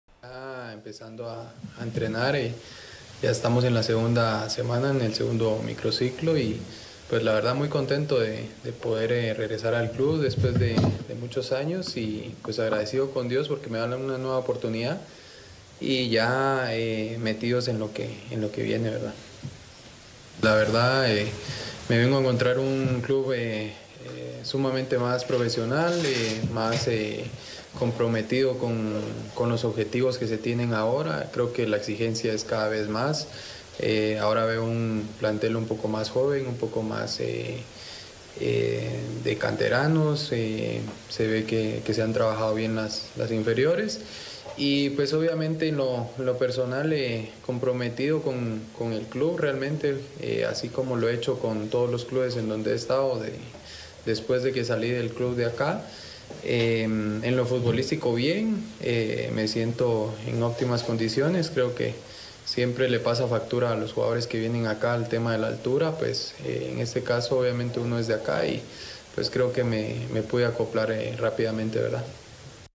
José Longo: "Regreso a un club que está más comprometido con sus objetivos"
El delantero dio sus primeras impresiones tras regresar a Xelajú MC después de muchos años.

José Longo se dirigió a los medios y aprovechó la oportunidad para destacar los trabajos que viene realizando Xelajú MC con sus canteras.